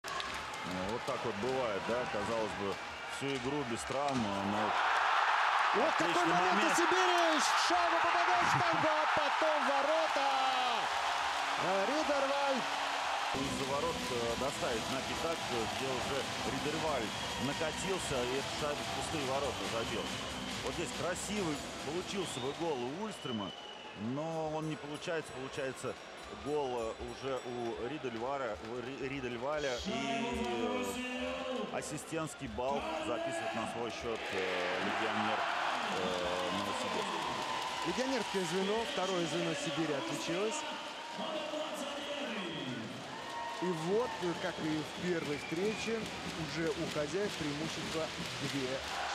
Sibir international line collective scoring work
Calle Ridderwall scores after Tomas Vincour and David Ullstrom almost render a masterpiece